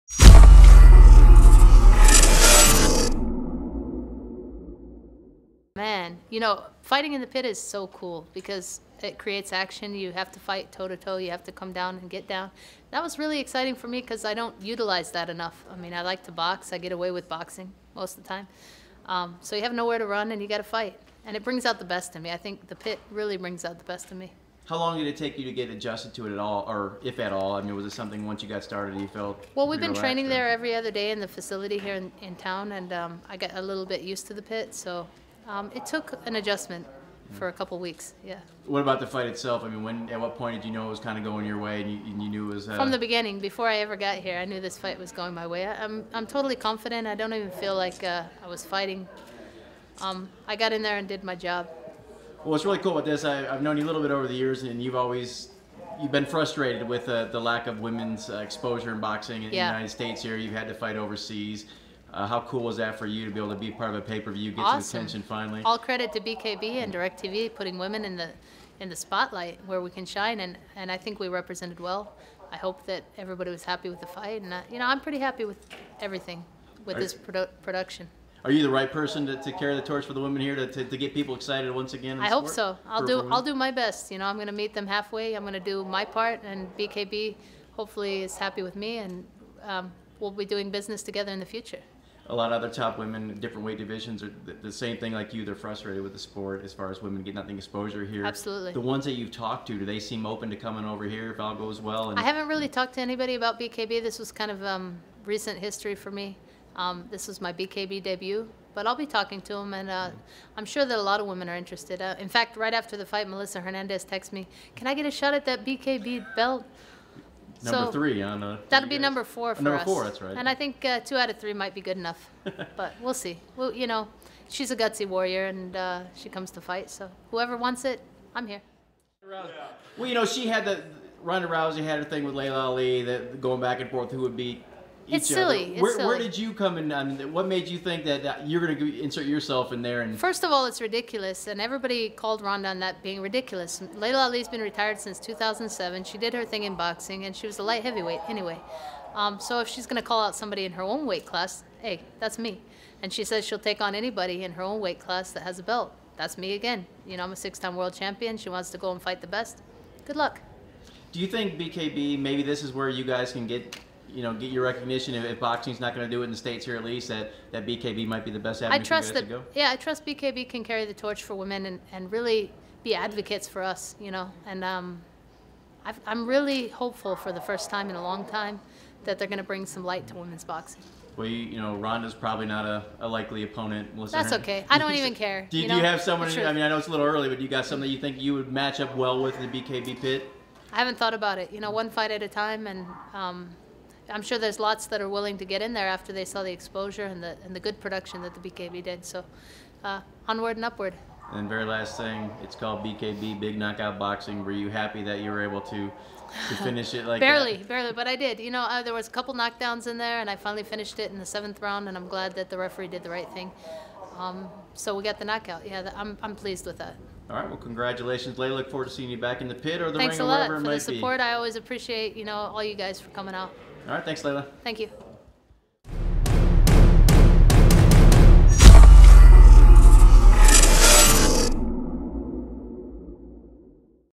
Layla McCarter on being first female BKB champ, Ronda Rousey comments, more
Following her dominating performance at BKB 2, where she became the first ever female champ for that organization, we spoke to Layla McCarter about her performance, whether or not this will lead to a resurgence in women's boxing, and of course her comments on Ronda Rousey